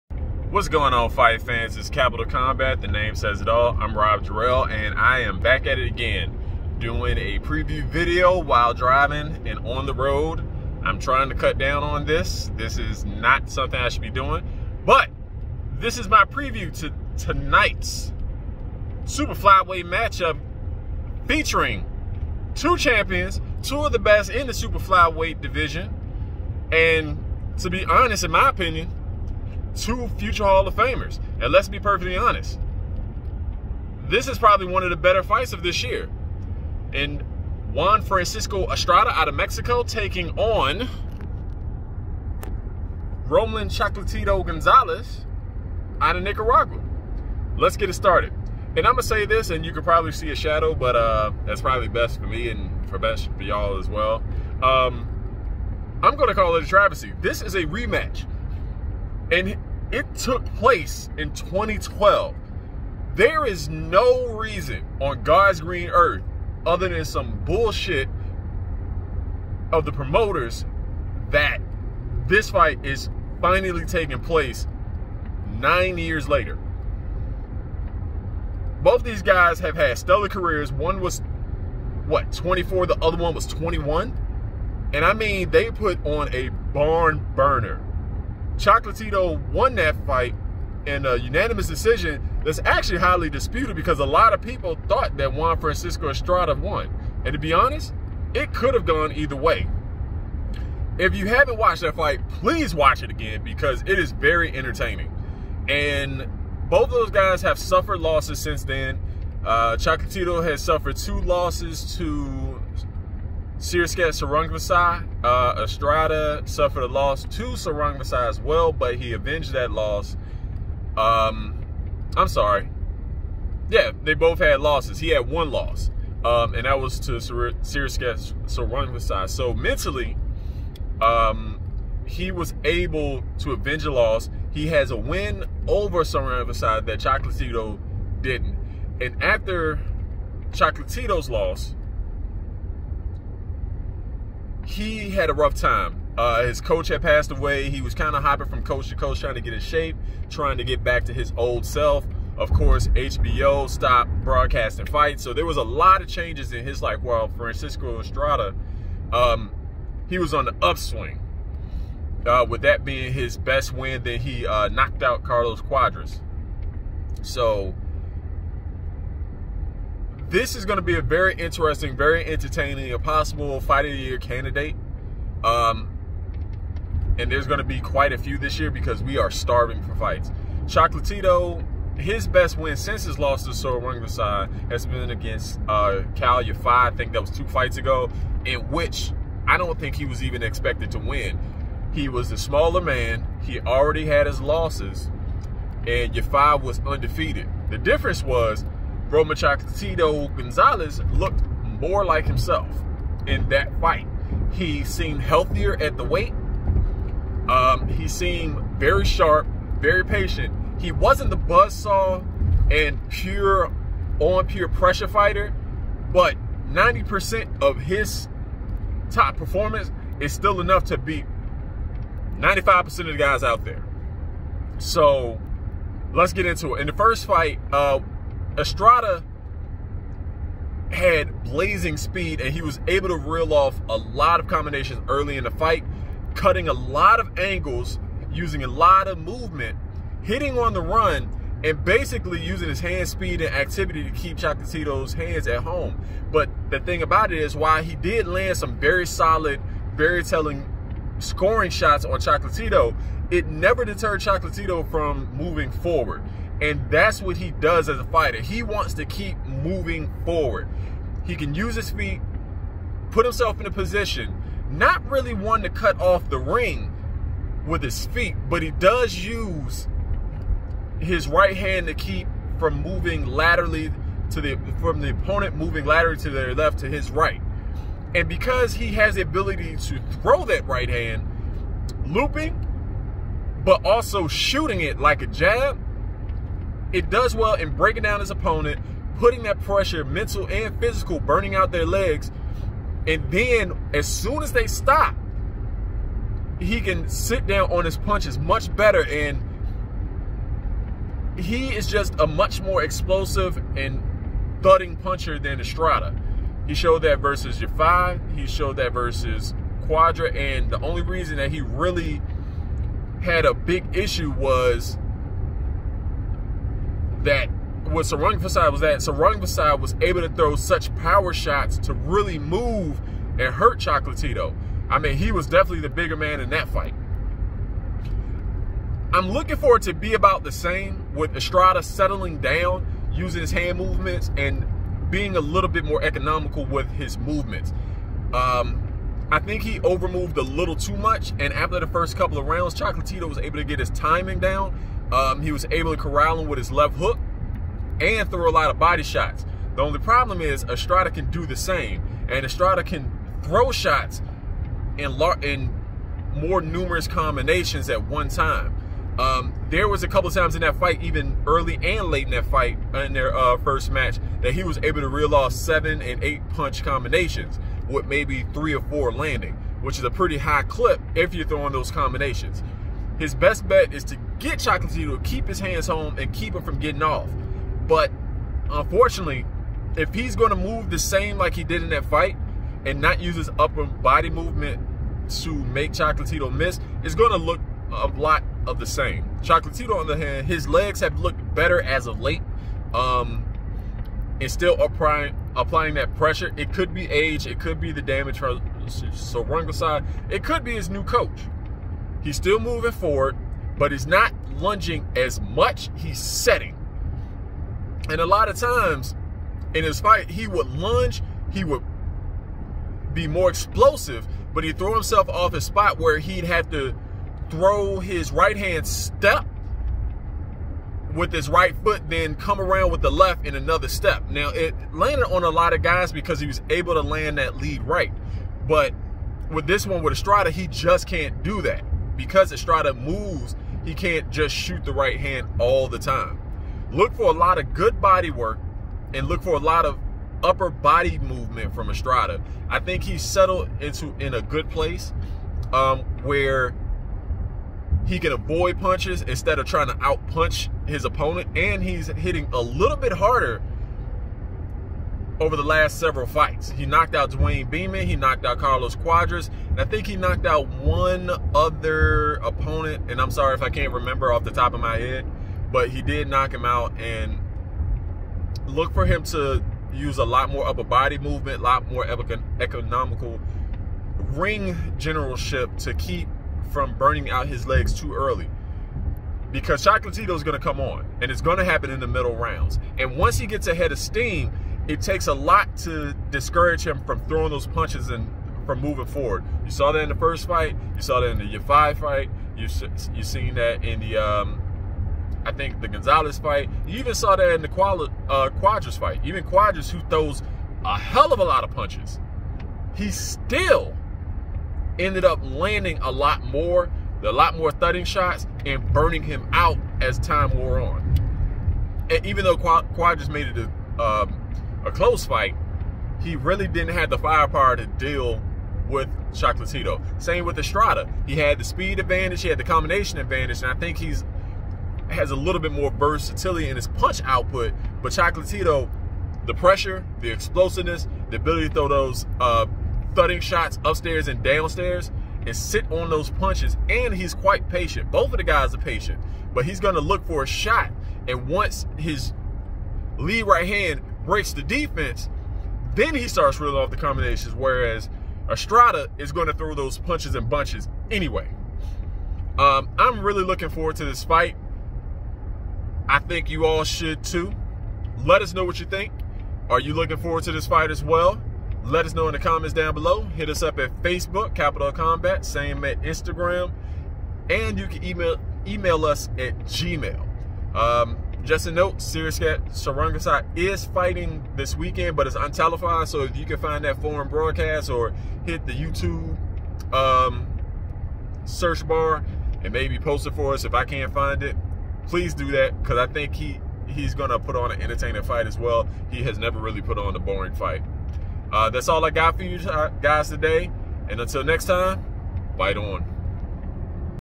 Superfly Rematch! Chocolatito takes on Estrada
Rob impromtu previews the long awaited and overdue rematch taking place between Roman "Chocolatito " Gonzalez and Juan Francisco Estrada in a Super flyweight tilt for the WBC, WBA and Ring titles